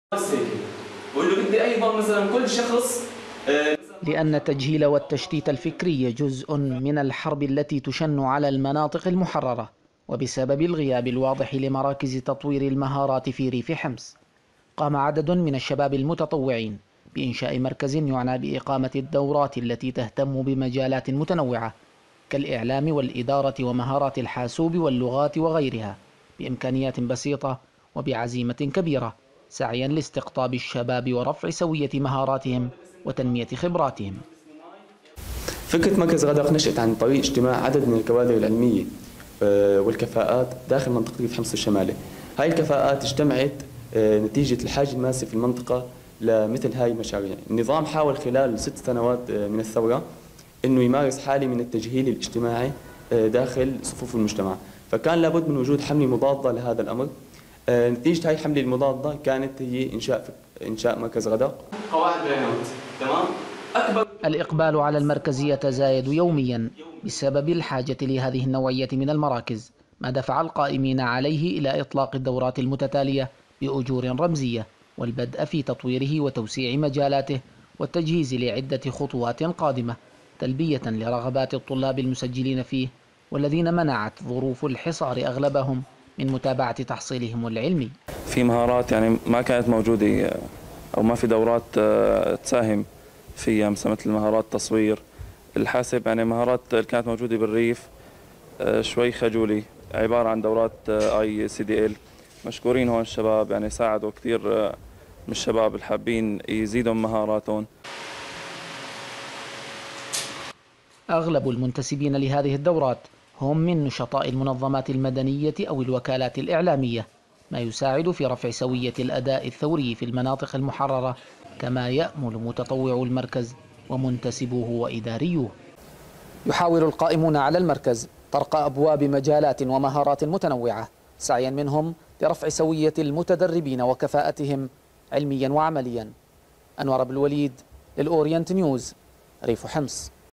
انشاء مركز تدريبي في مجالات فكرية ومهاراتية متنوعة
قام عدد من المتطوعين بإنشاء مركز يعنى بإجراء الدورات التدريبية في مجالات فكرية ومهاراتية متنوعة ويأتي افتتاح المركز بسبب غياب مراكز الدورات وانقطاع الكثير من الشباب عن دراستهم الجامعية بسبب واقع الحصار بريف حمص.

انور ابو الوليد / مراسل أورينت نيوز - ريف حمص